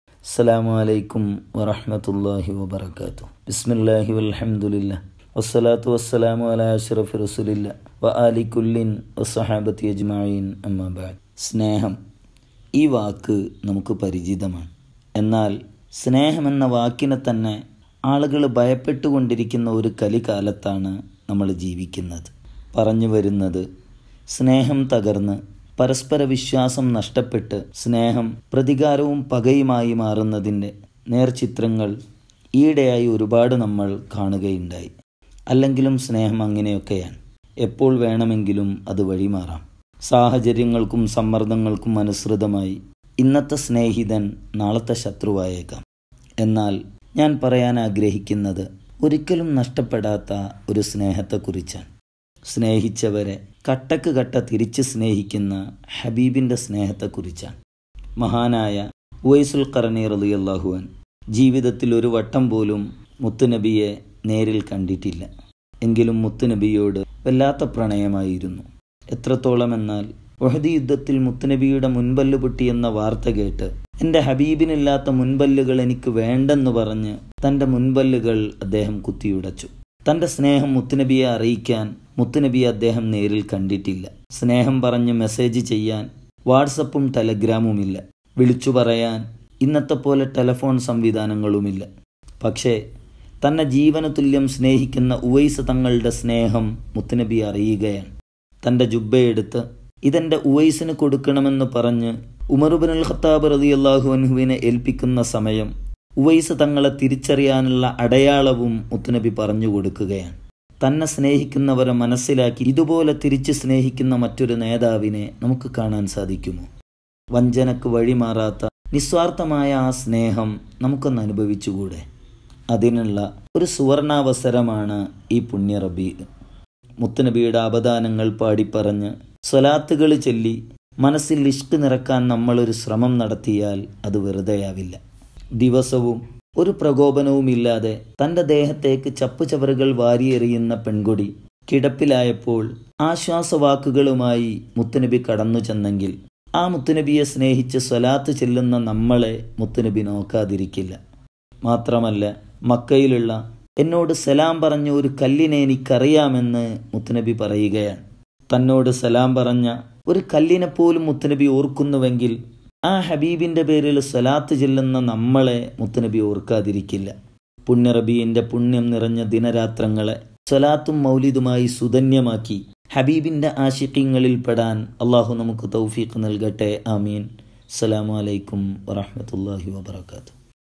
Isn't this love? | Uvais saqafi | sneham
ഒന്ന് സ്നേഹിച്ചു നോക്കു..., നമ്മുടെ മനസ്സിലെ സ്നേഹം നമ്മൾ പറയാതെ തന്നെ തിരിച്ചറിഞ്ഞ് സ്നേഹം കൊണ്ട് നമ്മെ തോൽപ്പിക്കുന്ന നേതാവാണ് മുത്ത് നബി